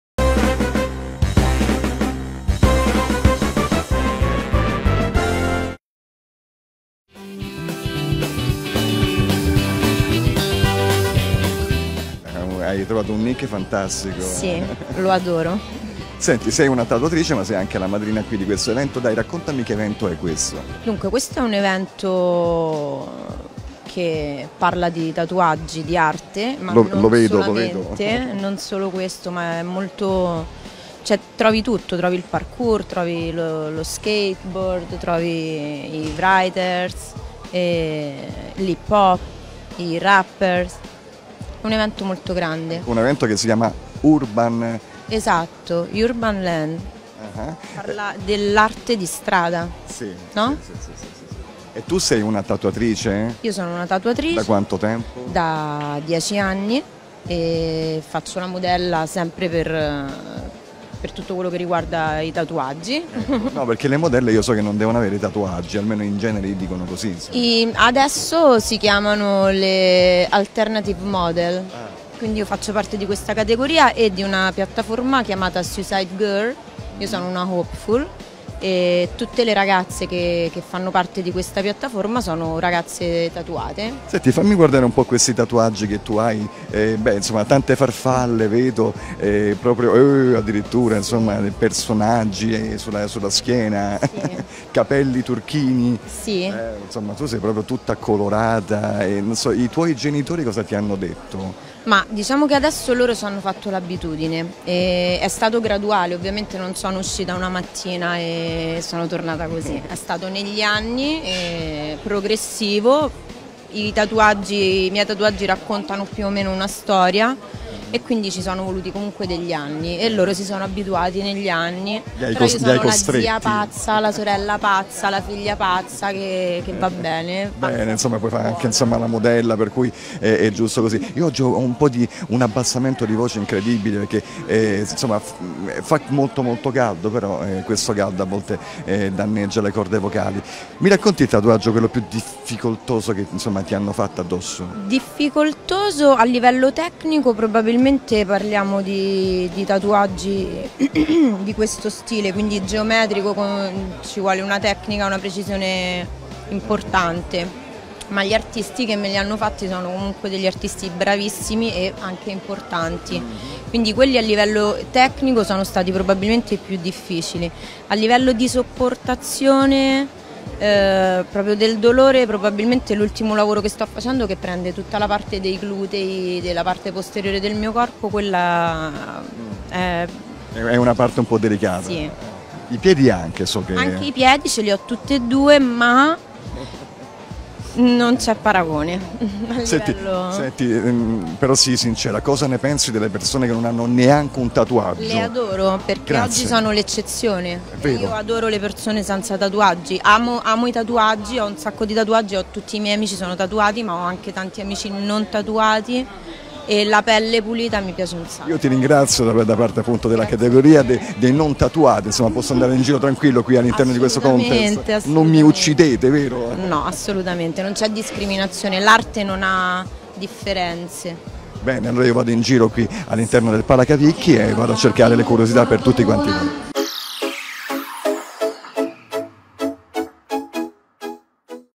Itervista a MISS Ink HALABAMA - Urban Land Tattoo Convention018
Urban Land Tattoo Convention018 - Palacavicchi ROMA
Itervista MISS Ink HALABAMA Alessandra Vitulano

Per MXO EVENTI situazione molto caratteristica :
Urban Land Tattoo Convention - per questo selezionato da noi per la nostra playlist Best Video
Chiaramente non neghiamo che è già da tempo che siamo rimasti ammirati dalla bella Alessandra Vitulano
seguiteci su - MXO EVENTI musica & cultura
Una playlist dedicata ai migliori video selection : caratteristica dell'evento e il fascino di Alessandra Vitulano MISS Ink HALABAMA